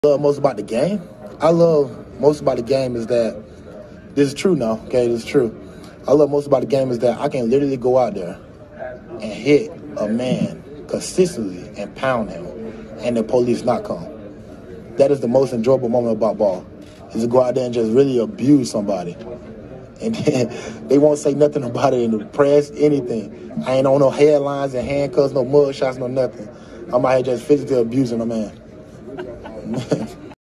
Why Marlon Davidson loves football
A rare four-year starter for Auburn’s defensive line, Marlon Davidson was asked what he loves most about the game of football at the 2020 NFL Combine.